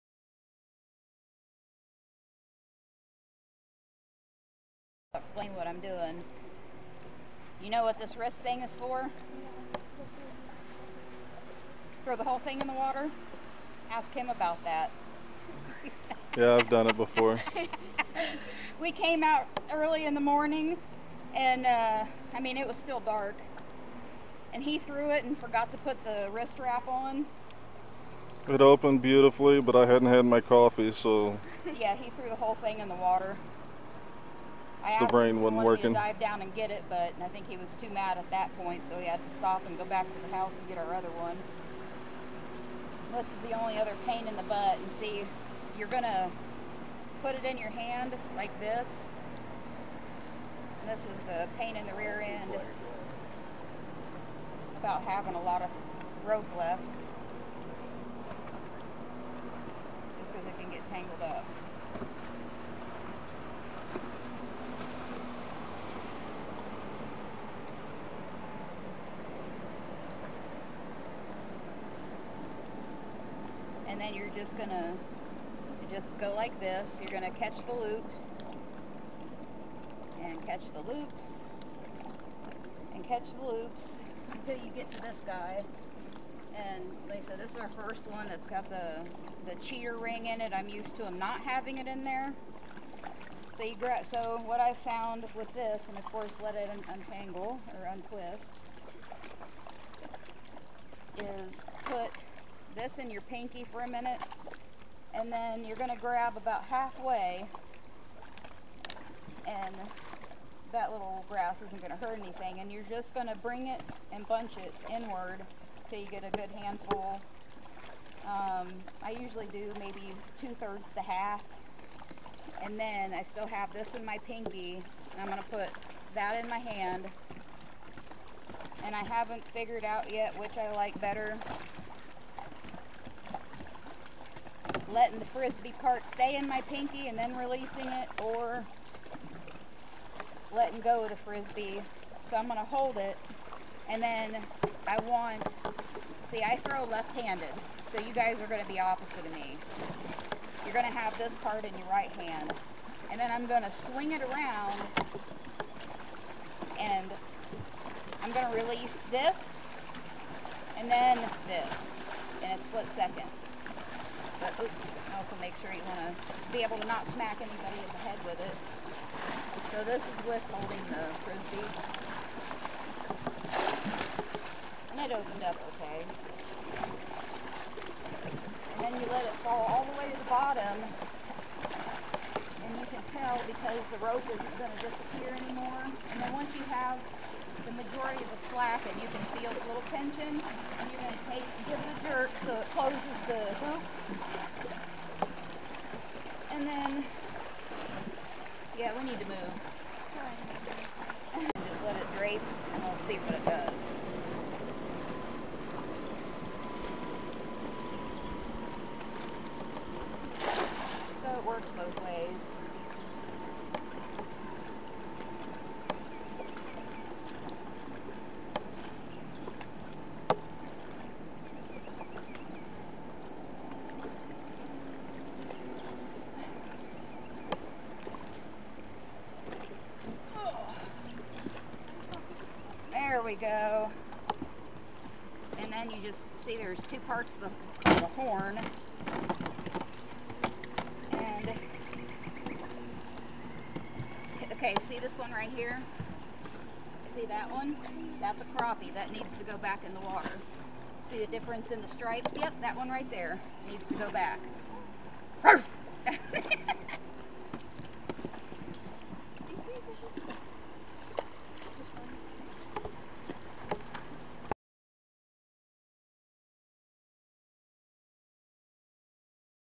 How to throw a casting net
My wife Melissa explains how we catch shad by throwing a casting net.